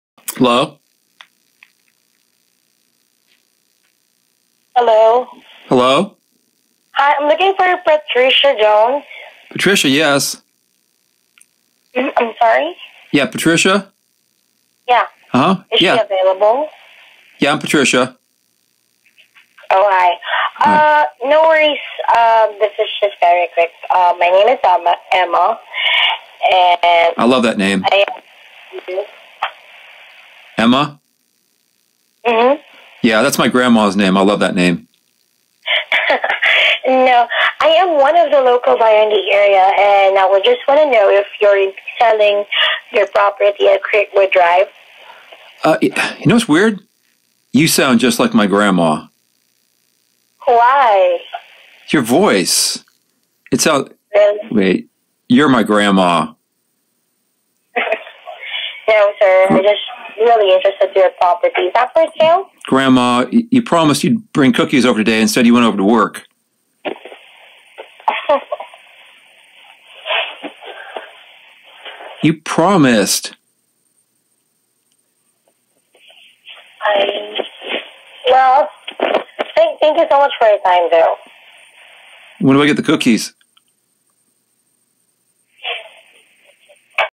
Grandma!
What happens when you suspect the telemarketer is your grandma?